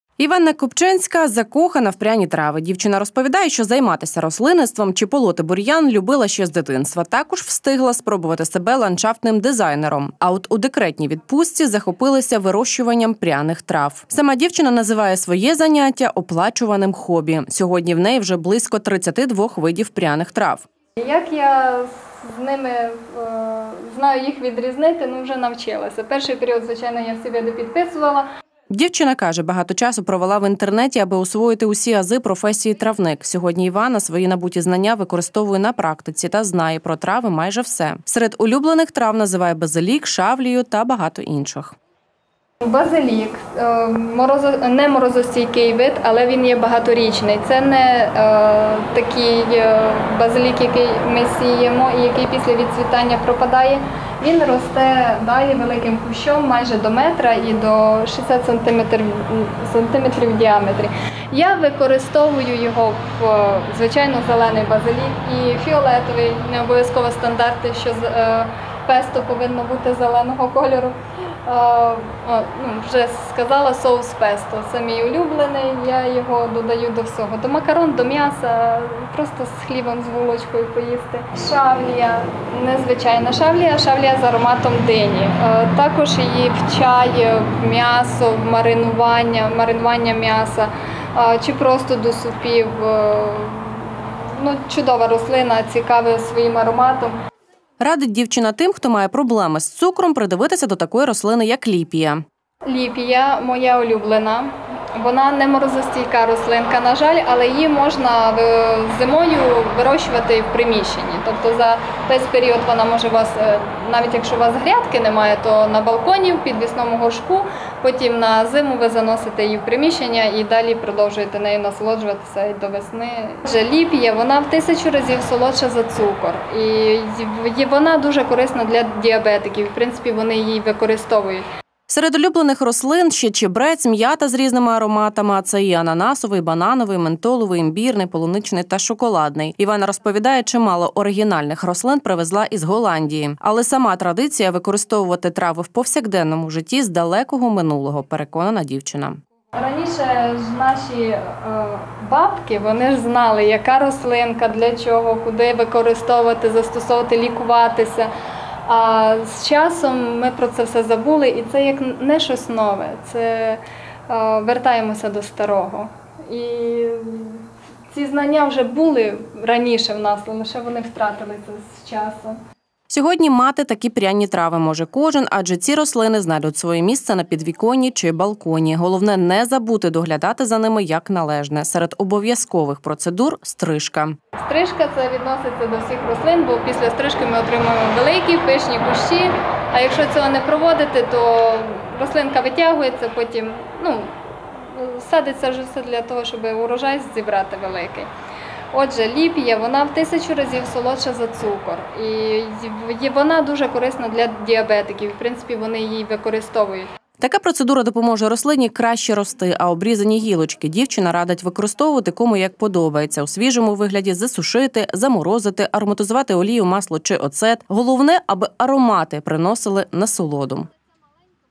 Світ пряних трав
Жити пряно і відчути всі аромати трав. Сьогодні вирощувати на підвіконні чи в городі орегано, базилік, м’яту чи мелісу може кожен. Але мало для кого ця справа стає справою життя. Серед тих, кому аромати та властивості трав приносять не тільки задоволення, але й прибуток – Іванна Купчинська. Журналістам “М-Студіо” дівчина розказала, як правильно доглядати та пряними травами та з яких рослин можна зробити власну ароматну оазу на підвіконні.